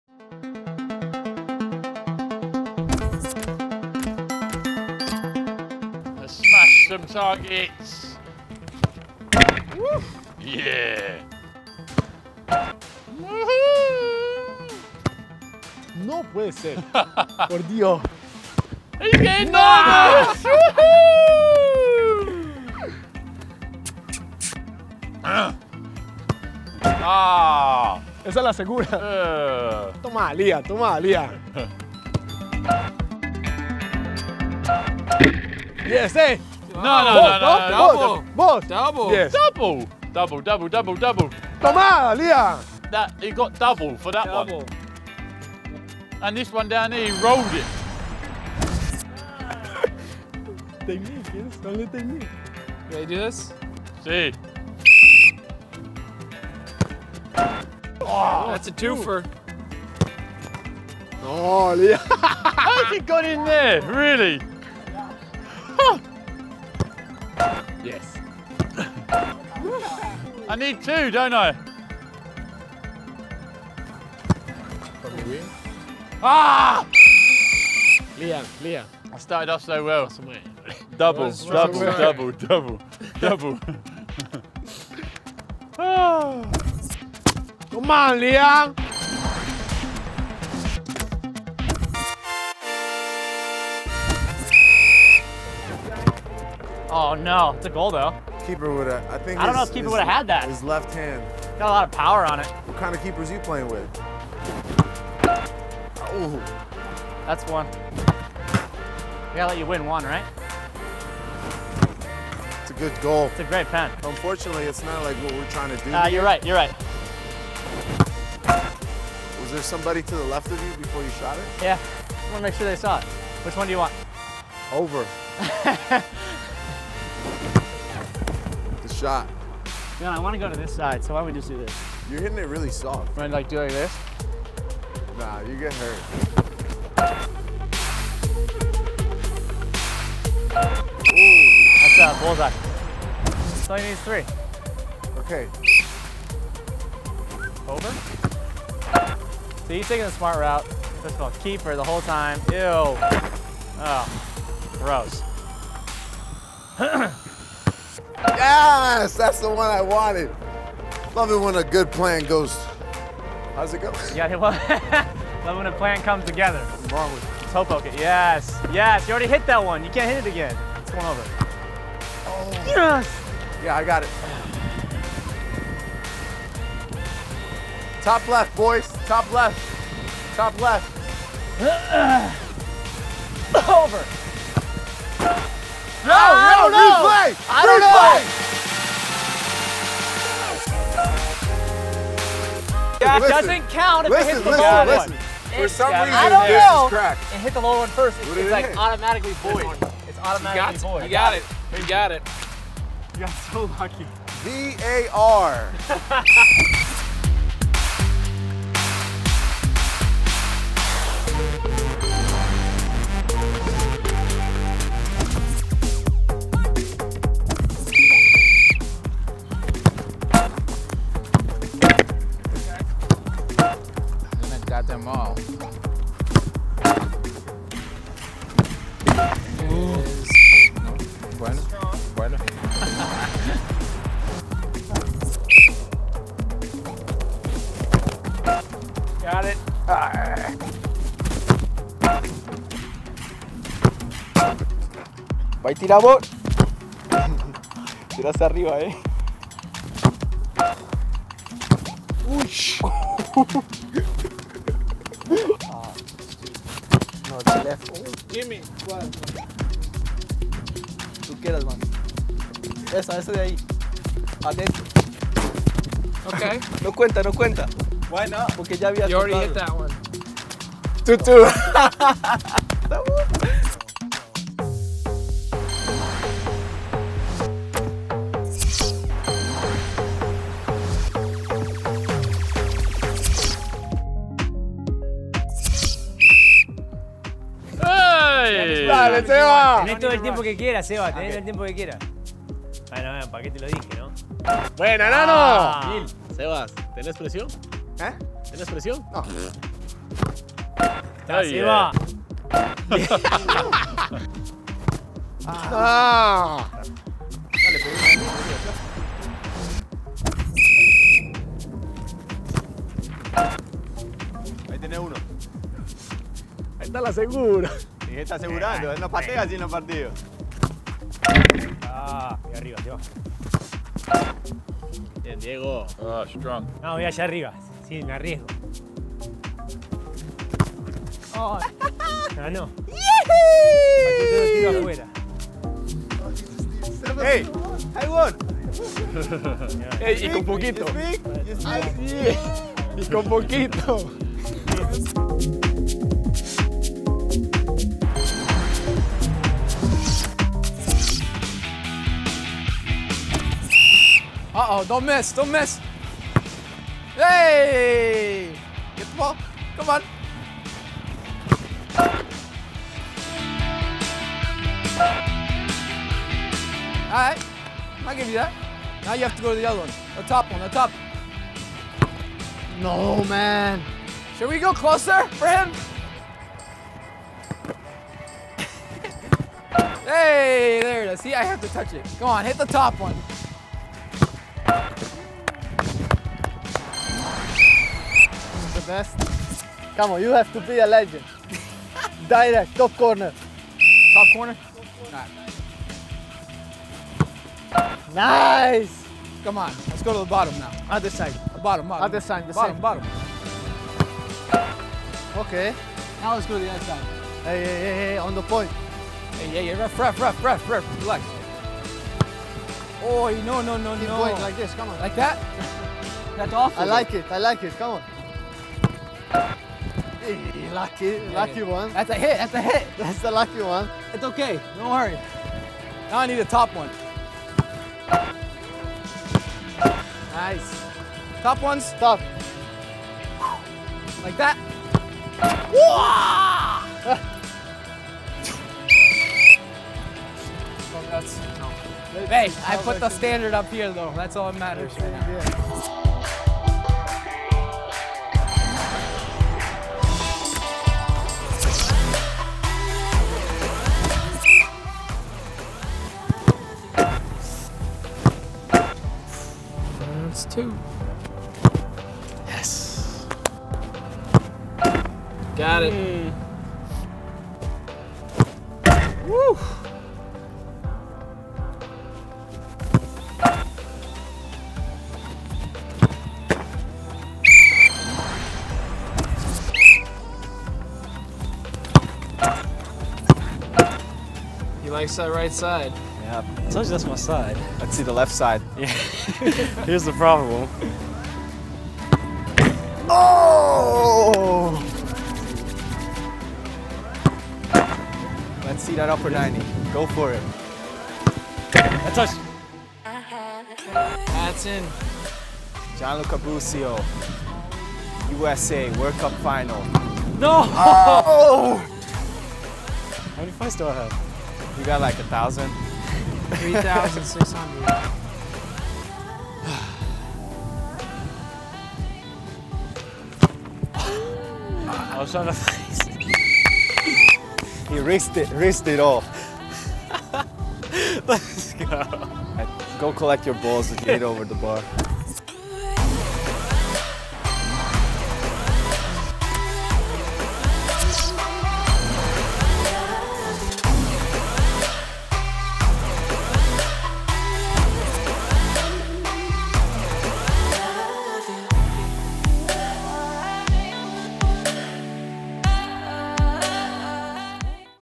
FIFA 18 Real-Life Skill Games | Penalty Shootout
8 balls. 8 targets. Who's got what it takes from the penalty spot?

Diego Valeri, Jimmy Medranda, Diego Fagundez, and more compete in the EA Sports FIFA 18 Real-Life Skill Games -- find out who came out on top.

This season of EA Sports Real-Life Skill Games was setup during MLS pre-season in Tucson, AZ and features the Houston Dynamo, New England Revolution, Portland Timbers, and Sporting Kansas City.